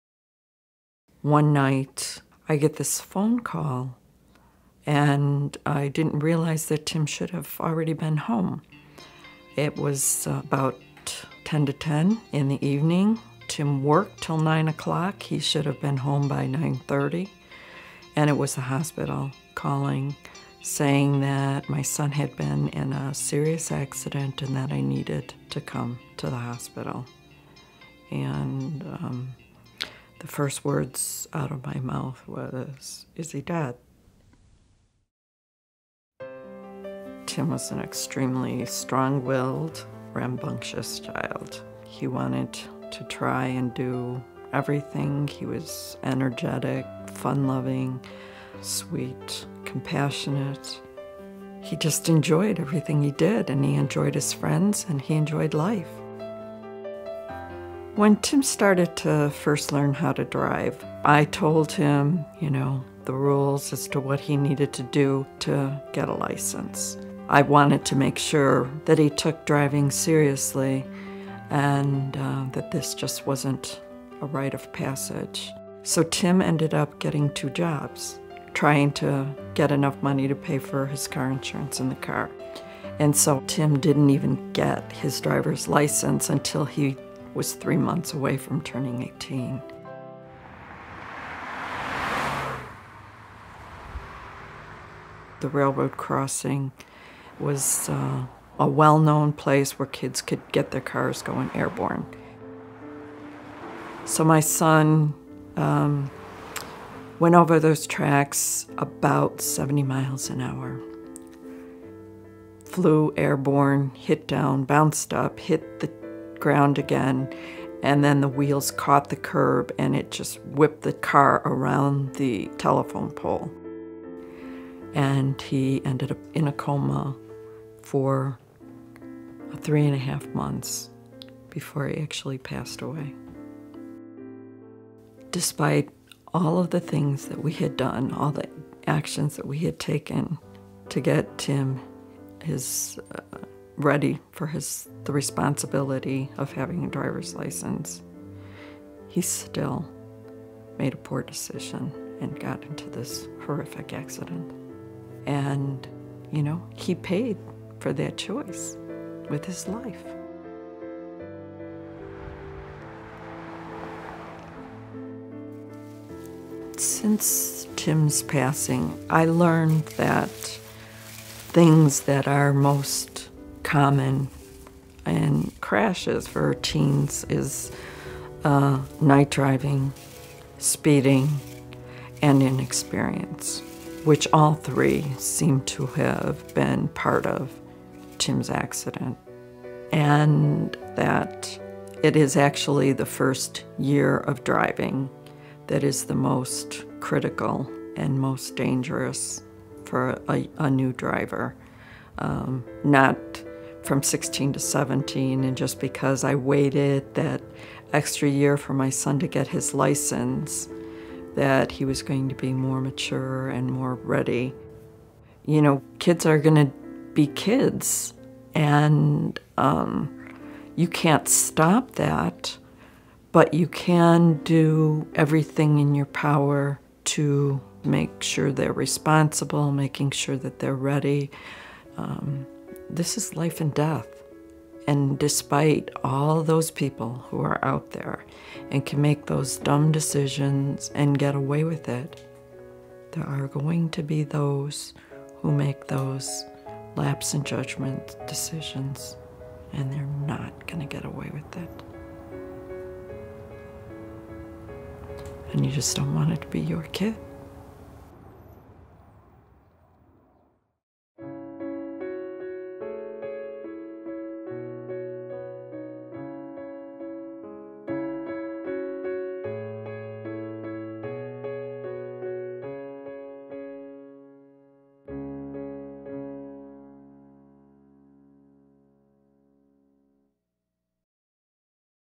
Tim's Story
A mom talks about how she lost her son - a teen who worked hard to earn the right to drive his own car - because of a bad choice he made in a split second.